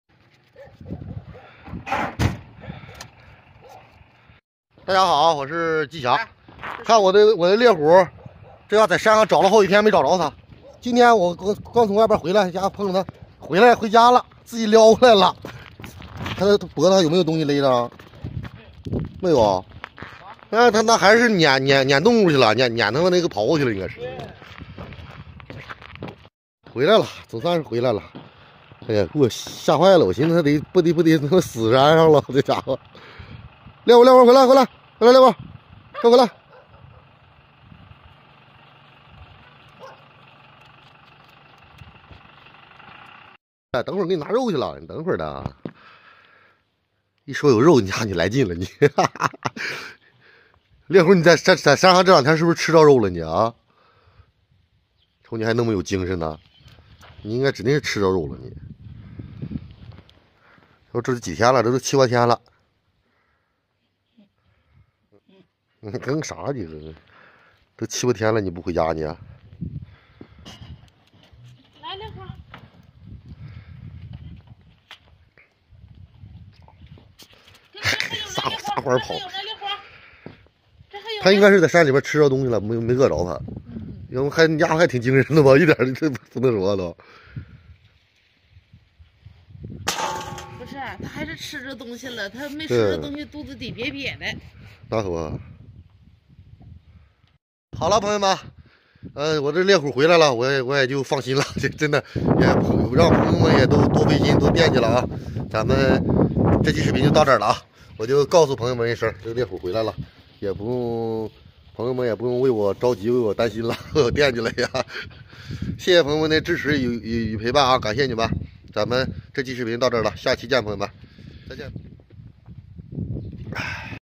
猎虎丢在山林7天回家了，还挺有精神，是不是在山里找到吃的了？
猎虎丢在大兴安岭山林7天了，今天回家了，它在山里找到吃的了？